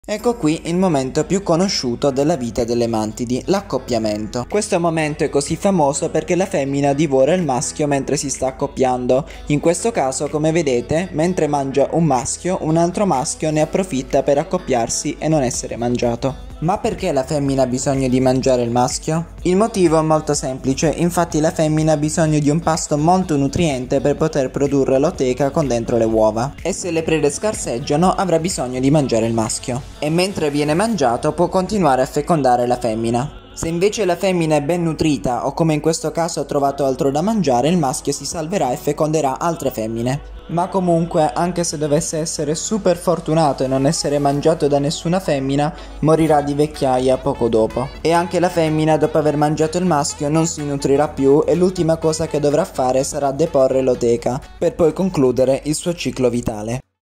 L'ACCOPPIAMENTO DELLE MANTIDI RELIGIOSE!
In questo periodo dell'anno le Mantidi hanno completato la metamorfosi e sono pronte ad accoppiarsi per poi morire.
Vi siete mai chiesti perché la Femmina mangia il Maschio?
 Ecco la risposta